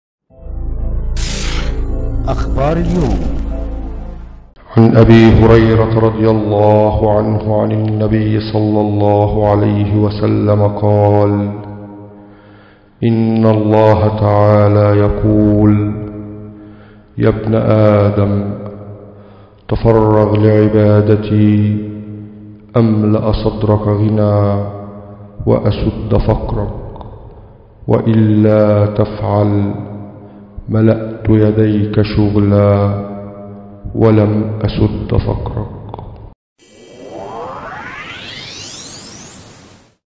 سلسلة الأحاديث القدسية .. قال تعالى " يا بن أدم تفرغ لعبادتى أملأ صدرك غنى "
عن أبي هريرة رضي الله عنه أن النبي - صلى الله عليه وسلم - قال : ( إن الله تعالى يقول : يا بن آدم تفرغ لعبادتي أملأ صدرك غنى وأسدُّ فقرك ، وإلا تفعل ملأت يديك شغلا ولم أسد فقرك ) رواه الترمذي وابن ماجة والإمام أحمد في مسنده وغيرهم ، وحسنه الترمذي وصححه الألباني .

عبادة الله هي المهمة العظيمة التي من أجلها خُلق الخلق ، وهي بمفهومها الشامل لا تقتصر على أداء الشعائر التعبدية - من صلاة وصيام وحج وذكر وغير ذلك - فحسب ، ولكنها تمتد لتنتظم حياة الإنسان كلها بشتى جوانبها وأنشطتها بحيث لا يخرج شيء منها عن دائرة التعبد لله رب العالمين ، وتمتد كذلك لتشمل جميع ما يحبه الله ويرضاه من الأقوال والأعمال الظاهرة

مدير التحرير
محمد جلال